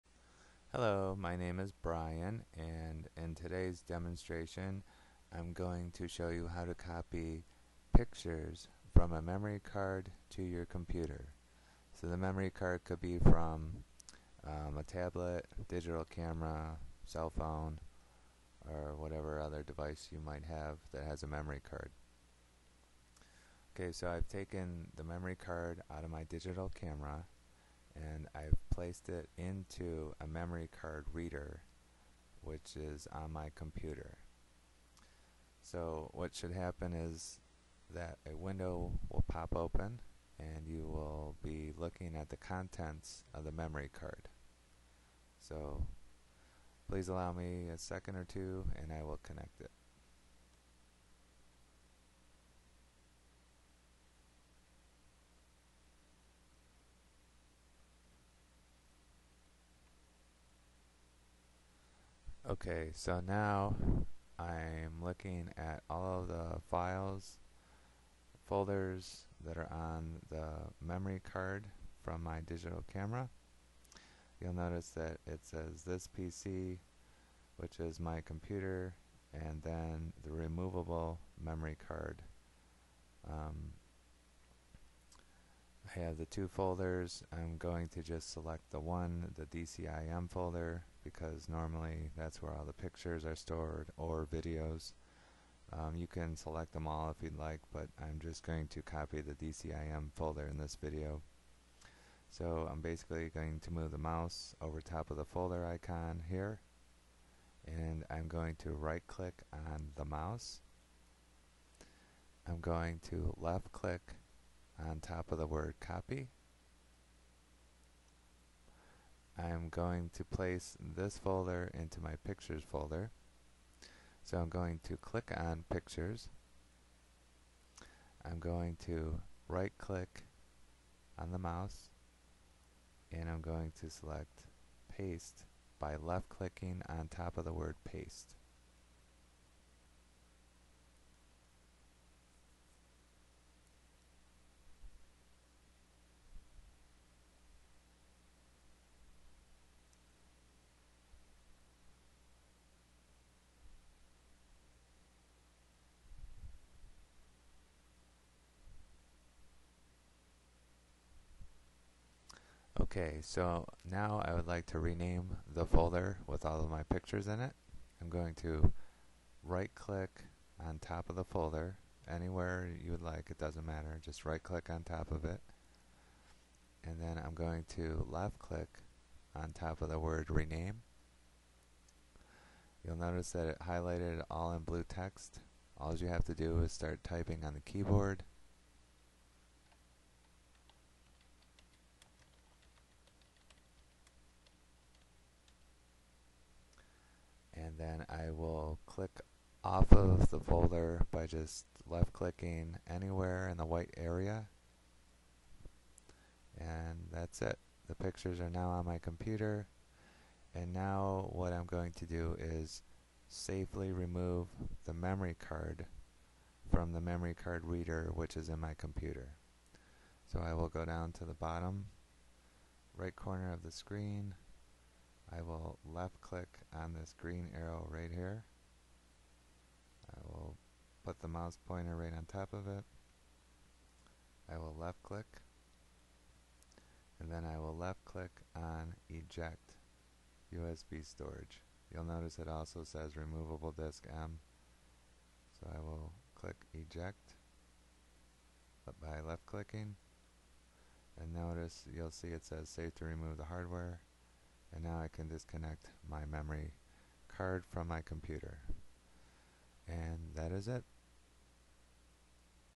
How to copy pictures from a memory card to your Windows computer
This video shows you how to copy pictures on a memory card to your computer.